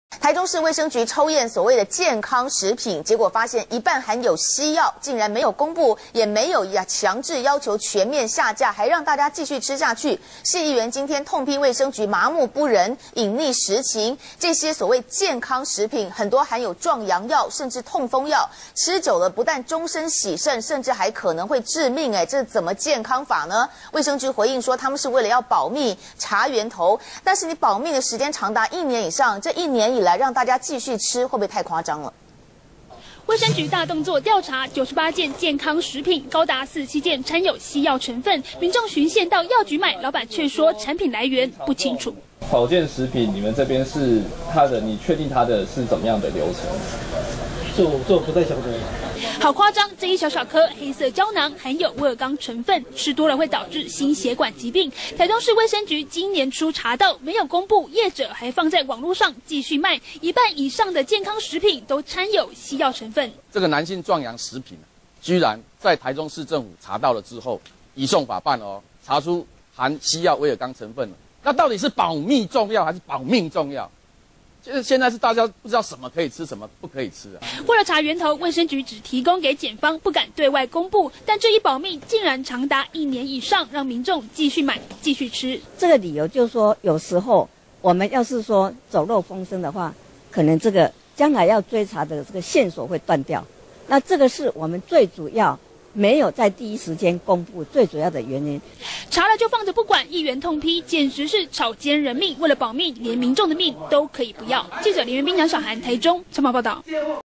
中天新聞》為查源頭拖一年！摻西藥食品民眾繼續吃
中天新聞》台中市衛生局抽驗健康食品發現，一半含有西藥竟然沒有公佈，也沒有強制要求全面下架，任由民眾繼續吃，市議員30日痛批衛生局麻木不仁，隱匿實情，這些健康食品很多含有壯陽藥，甚至是痛風藥，吃久了終身洗腎，還可能致命，對此衛生局回應，他們是為了要保密查源頭，但保密時間長達一年以上，會不會太誇張。